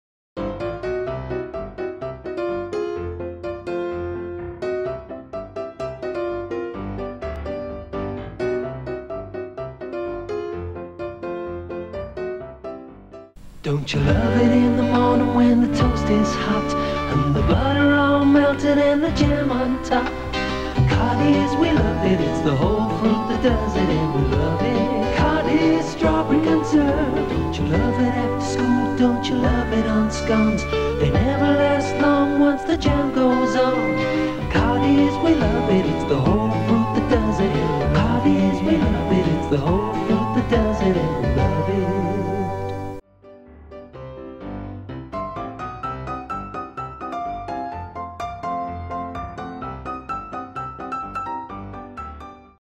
SRF127: COTTEES JAM 80s COMMERCIAL
Late 80s commercial for Cottee's Jam.